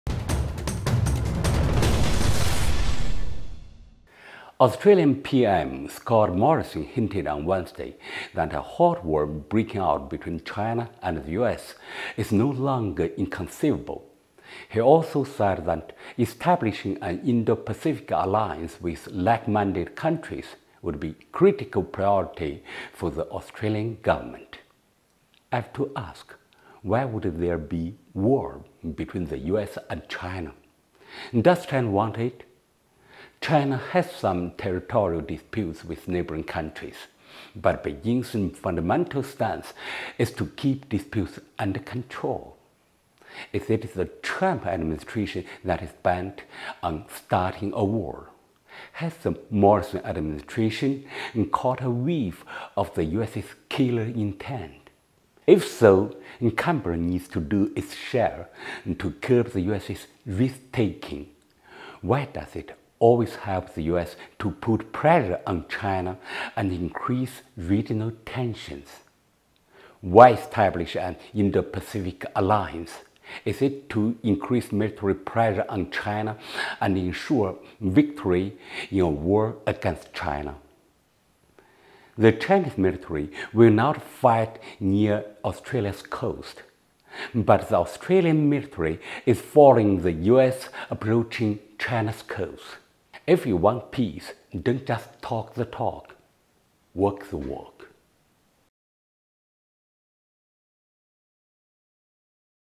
If Australia wants peace, walk the walk
The Chinese military will not fight near Australia's coast, but the Australian military is following the US, approaching China's coasts. If you want peace, don't just talk the talk. Walk the walk.